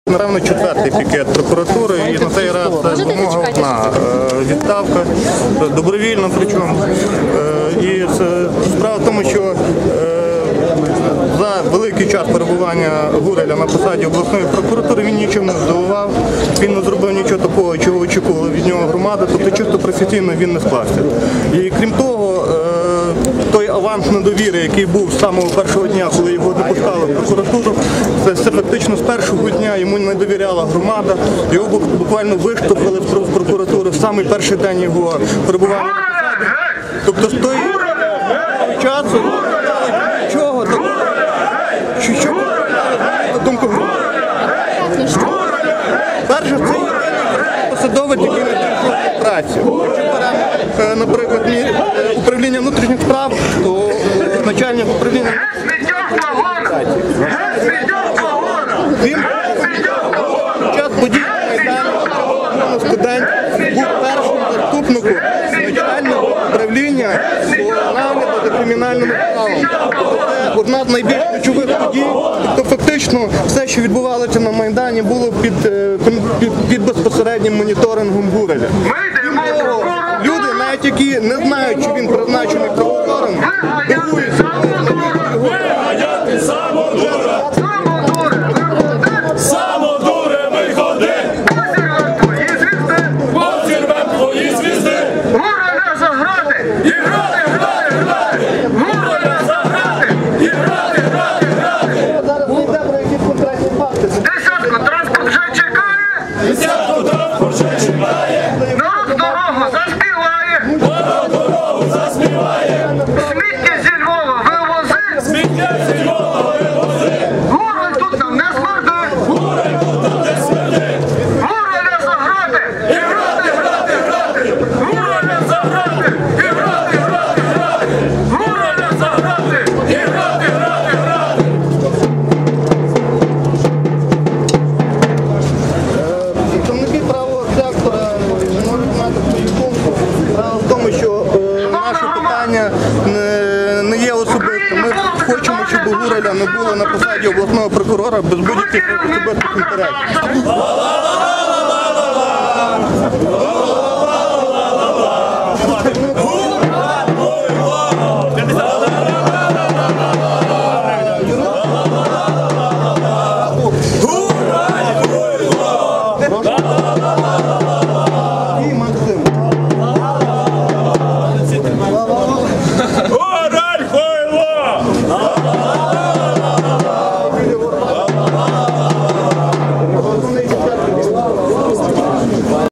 Білі Горвати пікетують Гураля речитативом
Пікет Львівської сотні самооборони "Білі Горвати" з вимогою негайної відставки прокурора Львівської області Володимира Гураля. .